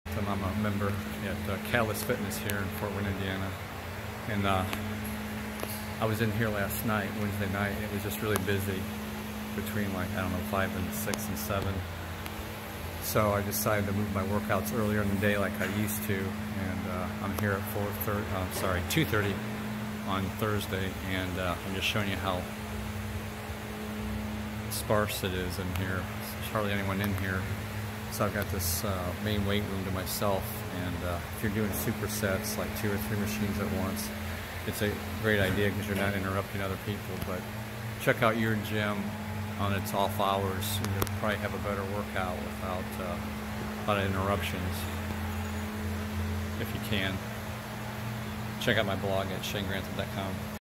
Catalyst Fitness NE Fort Wayne, IN. Off-hours workouts are less crowded.
I discuss crowded gyms and how light attendance is in the middle of the afternoon on a Thursday.  If you are fortunate enough to call your own schedule